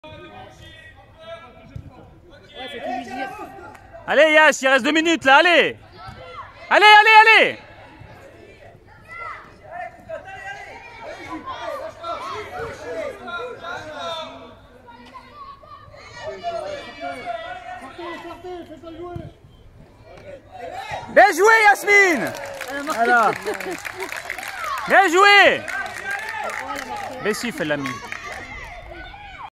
Olympique lyonnais u13 label
But de yasmine championnat u13 label
Face à dom tac 😉🥅👌⚽